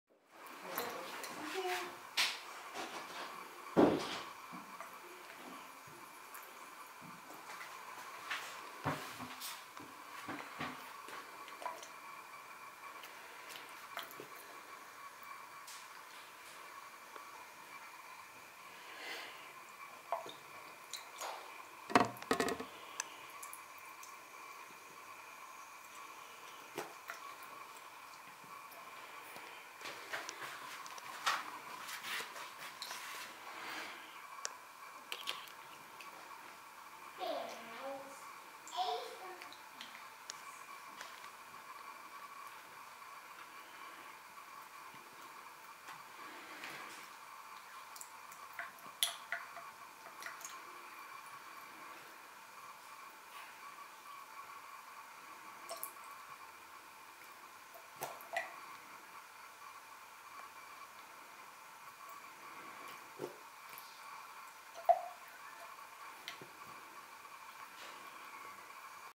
Cats obsessed with water
Oscar and Lucky are obsessed with water. When we go to the bathroom, they race in there hoping we'll turn on a trickle of water. They swat it, bite it, attack it, etc. The only problem is if you're sitting on the loo, they will sometimes flick the water onto you.